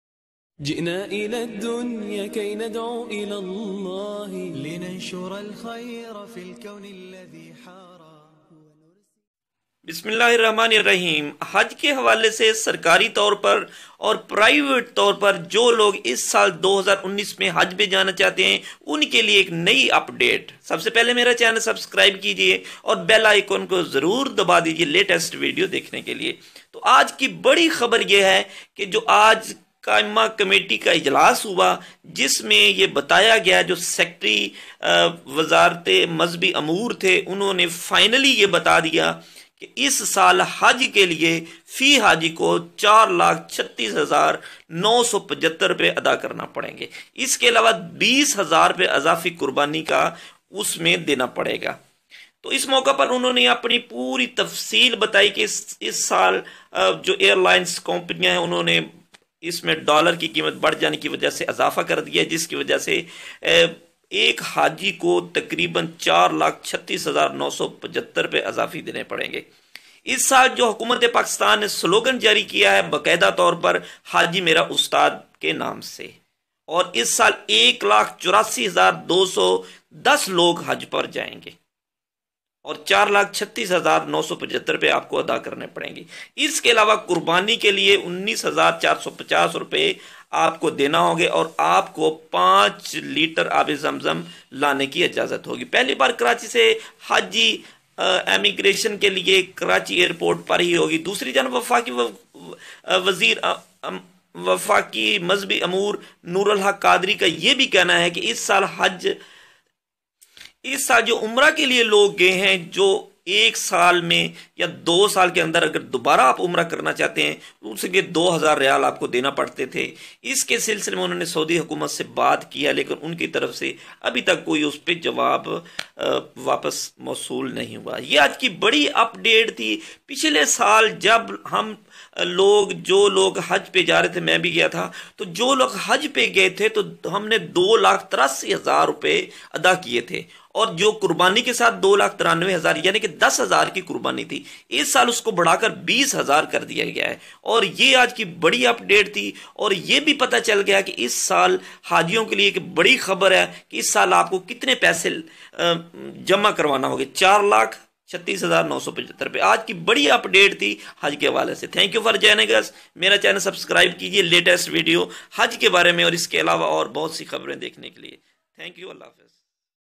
Today announced Policy Hajjh 2019 l Schedule of Govt hajjh scheme 2019 l Latest News Hajj 2019..
Islamic news Islamic information and all islami biyaan for Muslim brotherhood and sisterhood on Islamic lab TV.Today announced Policy Hajjh 2019 l Schedule of Govt hajjh scheme 2019 l Latest News Hajj 2019..